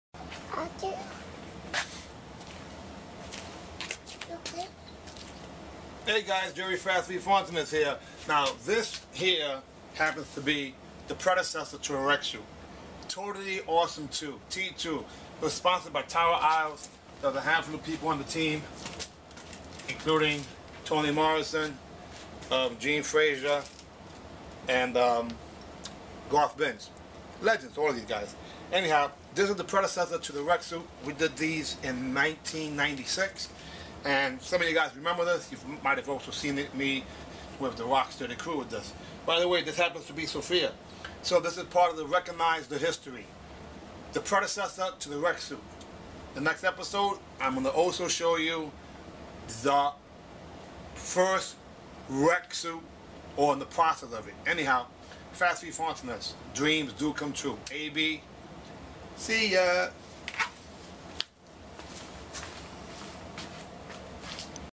Rek Suits #1,Rekonize Da History Vol. 1 of 10
Jerry"Fast Feet" Fontanez wearing the T2 (Totally Awesome 2)
1996 Team Uniform